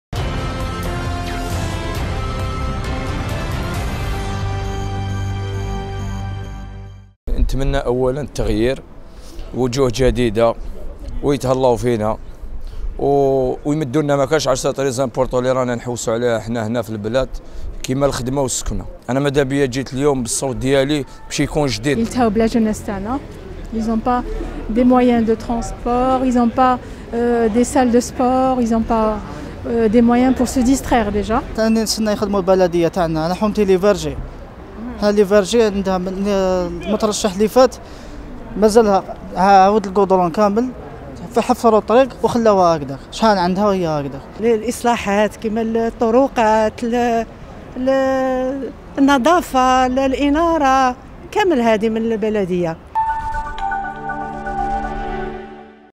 محليات 27 نوفمبر : تطلعات بعض الناخبين بالعاصمة من تجديد المجالس البلدية و الولائية
الجزائر (وأج) -  محليات 27 نوفمبر : تطلعات بعض الناخبين بالعاصمة من تجديد المجالس البلدية و الولائية